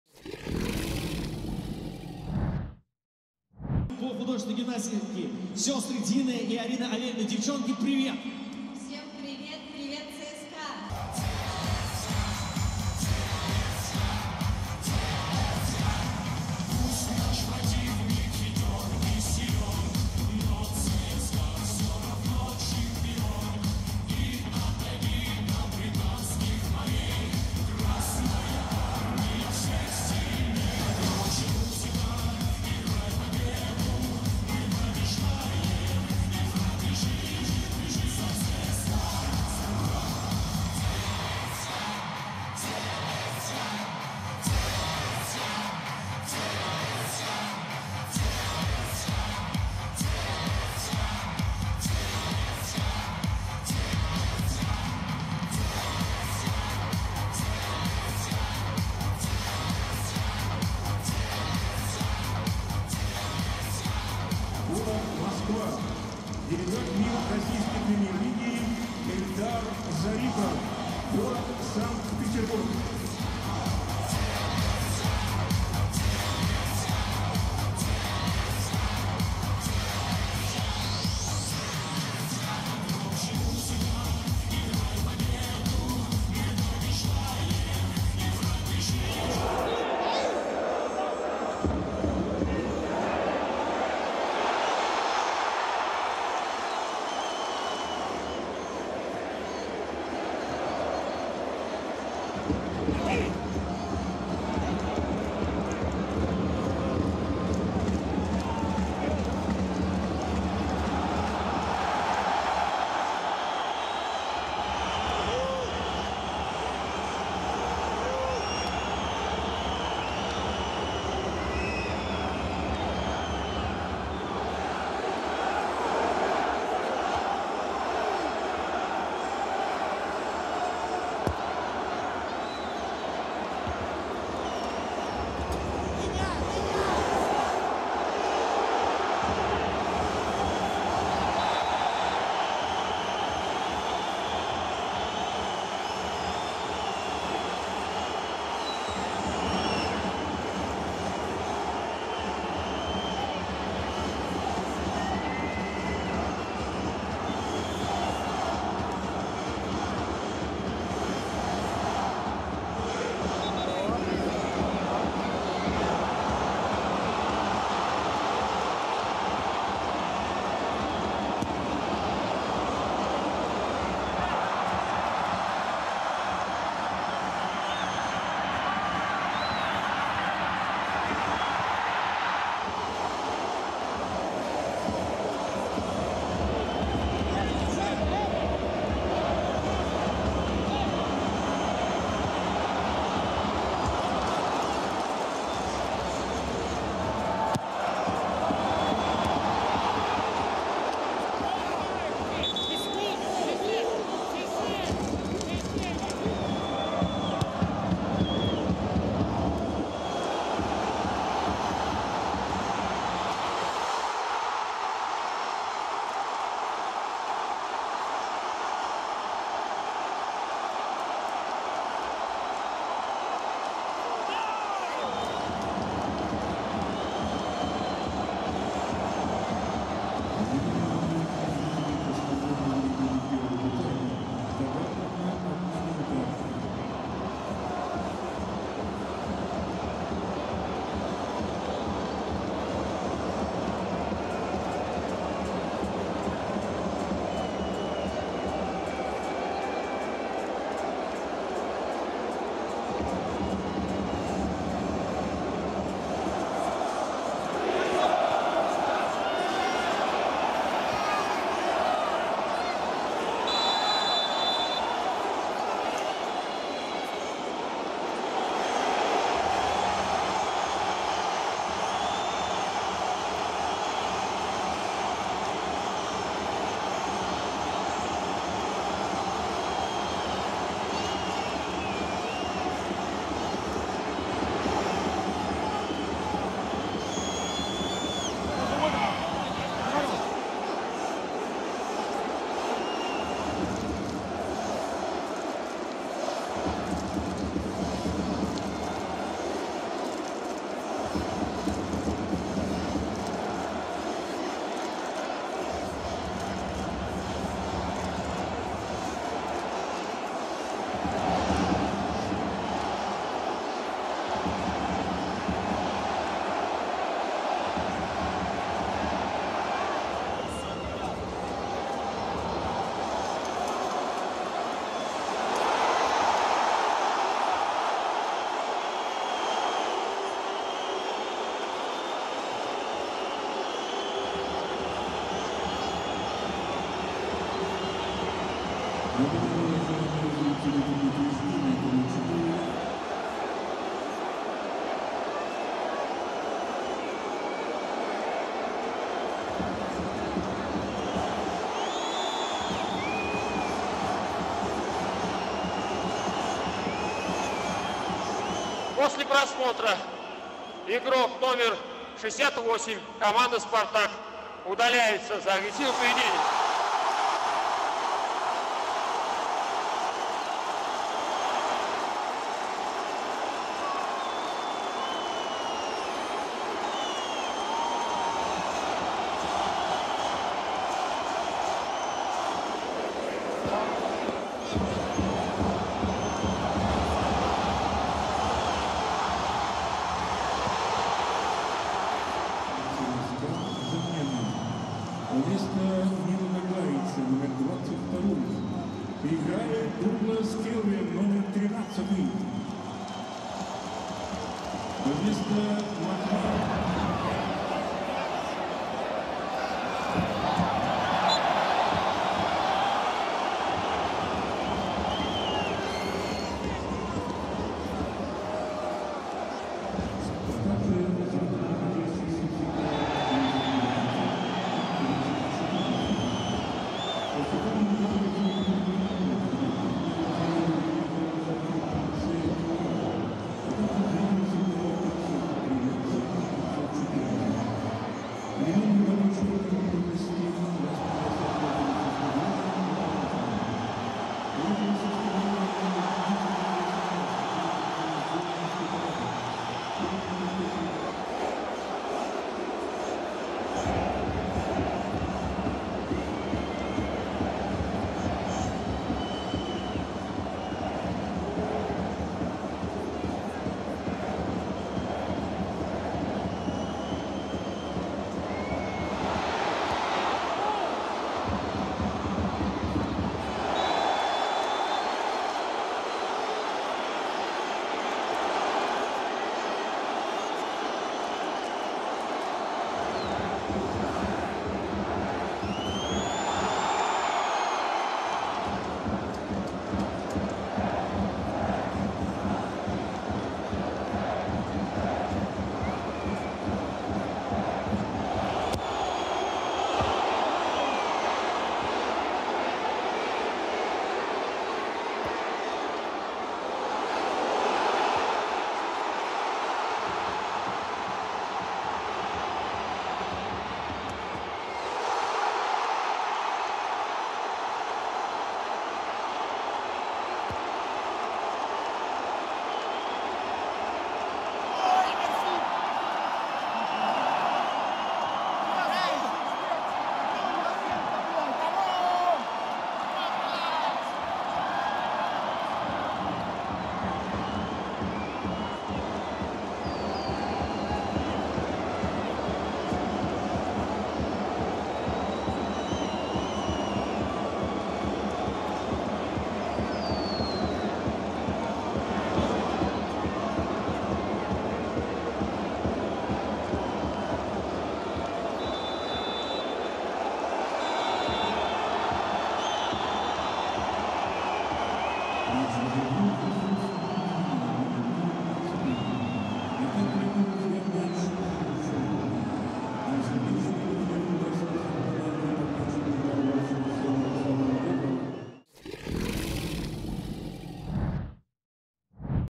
Обзор матча ЦСКА – «Спартак» | Мир РПЛ 2024/25
▶️ Смотри лучшие моменты Winline Матча тура ЦСКАСпартак в 14-м туре